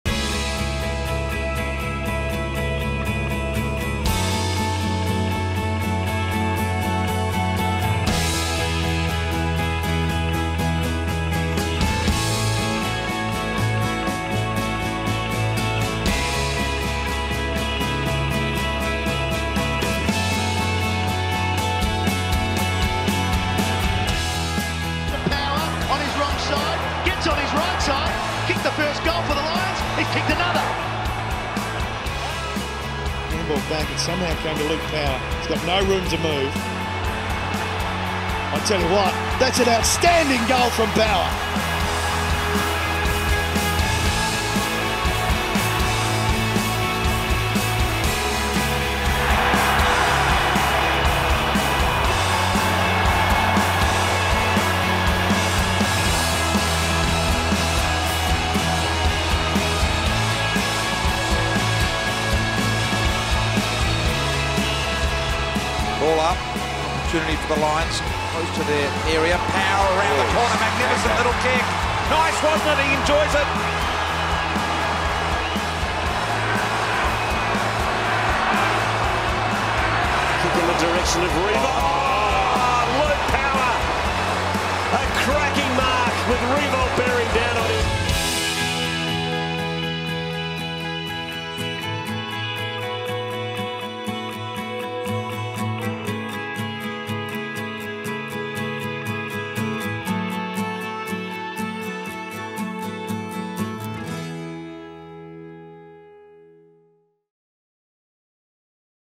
Luke Power - 300 AFL games
Luke Power is set to play his 300th game this weekend.